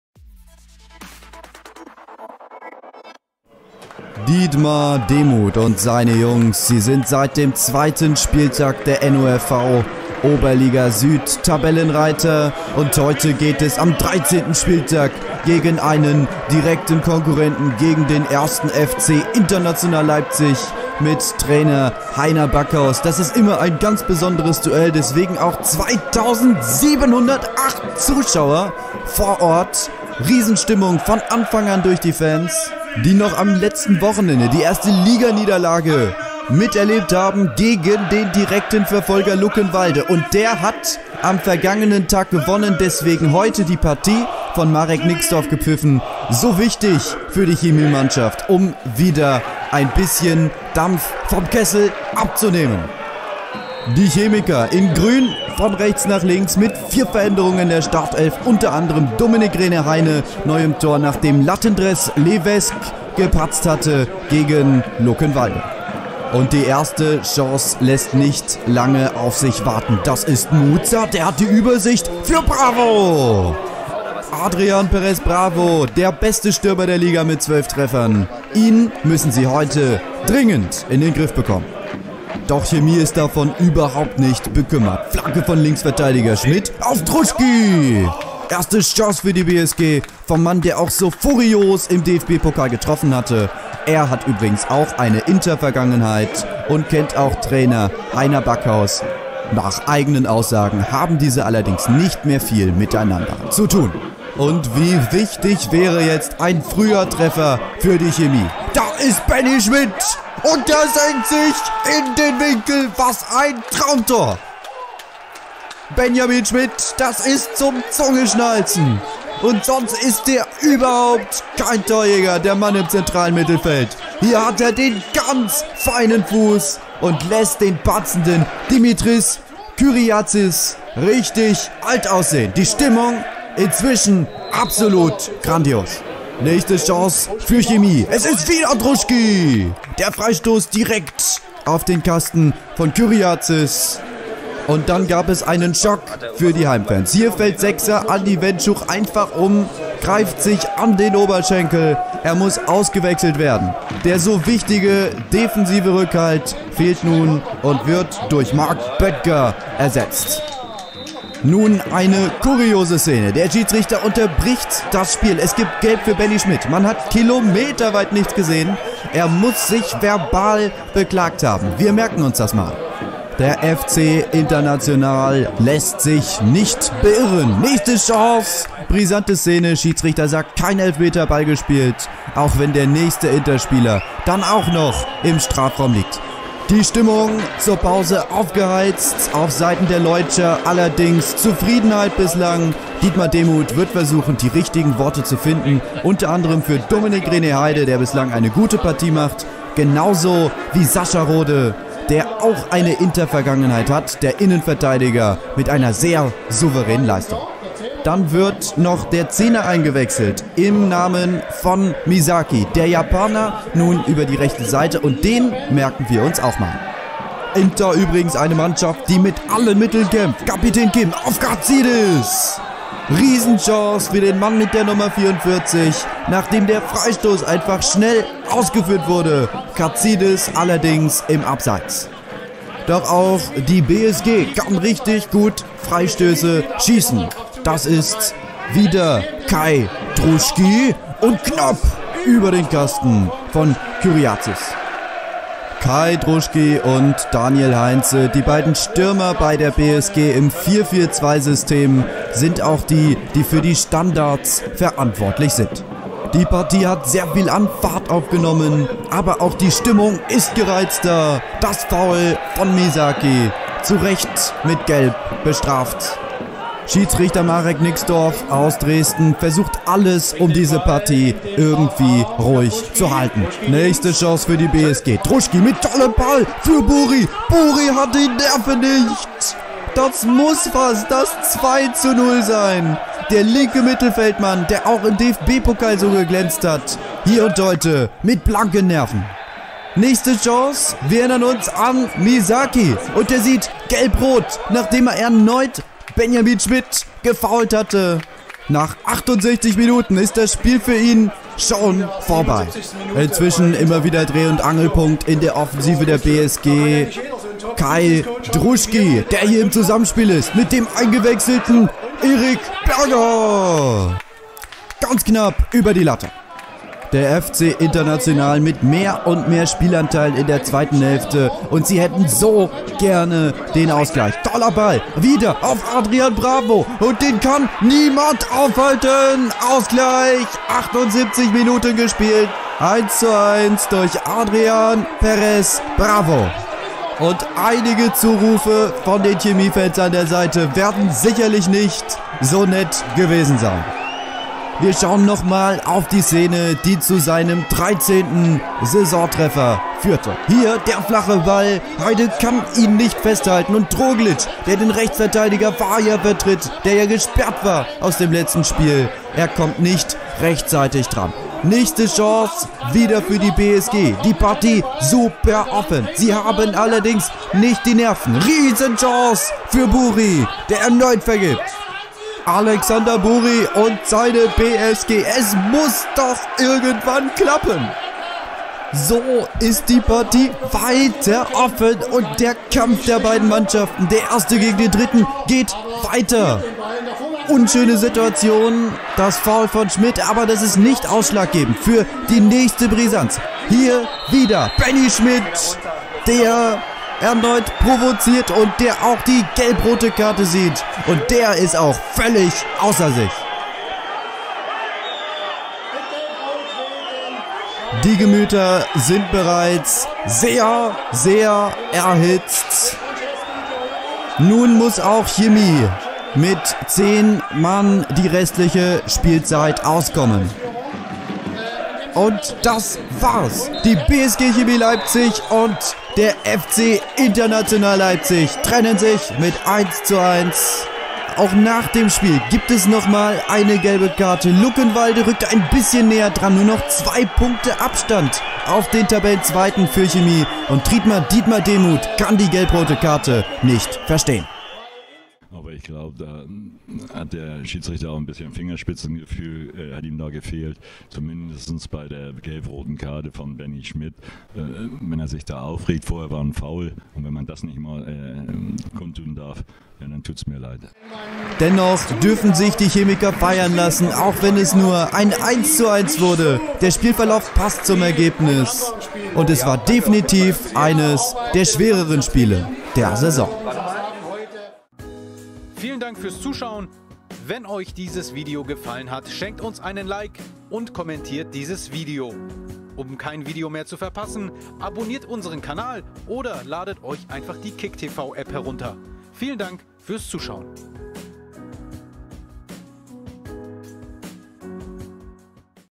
BSG Chemie Leipzig - FC International Leipzig (13. Spieltag, NOFV-Oberliga Süd)
Schmidt trifft und fliegt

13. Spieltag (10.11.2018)

Tore:
1:0 Benjamin Schmidt
1:1 Adrian Perez Bravo

Aufstellung BSG Chemie Leipzig:
Dominic-René Heine 16, Dominic-René Haine 02, Stefan Karau 03, Andy Wendschuch 06, Alexander Bury 07, Marko Trogrlic 08, Daniel Heinze 10, Benjamin Schmidt 13, Max Keßler 19, Lars Schmidt 39, Kai Druschky 77

Ersatzbank:
Julien Latendresse-Levesque 31, Niklas Opolka 05, Eric Berger 09, Marc Böttger 11, Sebastian Berg 15, Branden Garrett Stelmak 23 , Florian Schmidt 27

Trainer:
Dietmar Demuth

Aufstellung FC International Leipzig:
Dimitris Kyriatzis 99, Bastian Strietzel 05, Dongmin Kim 08, Christos Lereidis 12, Raymond Aschenkewitz 17, Robert Rode 18, Juan Jose Serrano De la Cruz 21, Adrian Perez Bravo 22, Tzonatan Moutsa 32, Marcelo Henrique Franceschi 39, Ioannis Kantartzis 44

Ersatzbank:
Ahmad Hellal Hosainie 01, Salvatore Paul Russo 04, Artem Cygankov 06, Christopher Sadamu Misaki 10, Zoran Levnaic 16, Skodran Zeqiri 37

Trainer:
Heiner Backhaus

Kommentator/Produzent:
Jan Ole Behrens